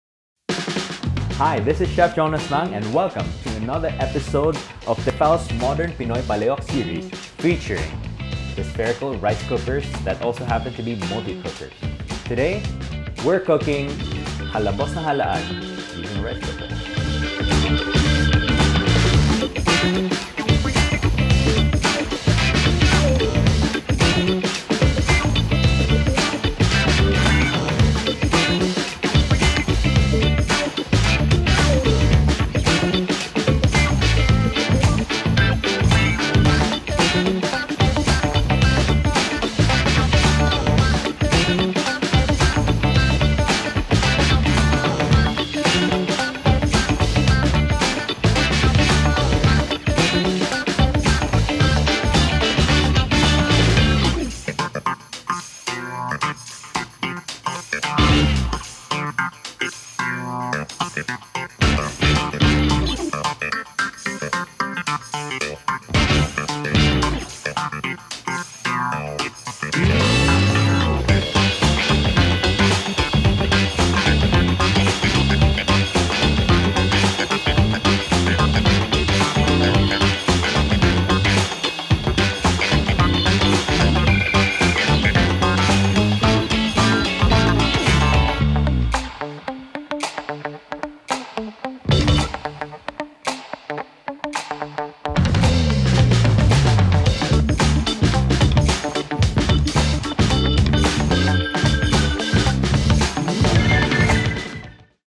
TEFAL SPHERICAL POT RICE COOKER: HALABOS NA HALAAN
We got your seafood cravings and your warm Friday nights covered all at once! Tune in as we teach you our light seafood soup, Halabos na Halaan, using the Tefal Initial Spherical Pot Rice Cooker!

Ingredients:
vegetable oil (1 tbsp)
clams (500 g)
ginger (sliced, 2 pcs)
cilantro root (cleaned, 2 pcs)
lemongrass (trimmed,  2 pcs)
fish sauce (2 tbsps)
calamansi juice (2 pcs)
red pepper (chopped, 2 pcs)
brown sugar (1 tbsp)
cilantro leaves (2 tbsps)

Procedure:
1. Pre-heat on Soup Setting. Sautee the ginger, cilantro root and lemongrass in oil until fragrant.
2. Add the clams and sautee for a couple of mins, then add the water, fish sauce, calamansi juice and brown sugar.
3. Close and cook until most of or all the clams are open (25- 30 mins).    4. Season to taste, and garnish with cilantro leaves.

Tips! Soak in salted water for 1-2 hours to purge sand, then rinse and discard open ones.
Note: Recipe can be done with other seafood like mussels or shrimps.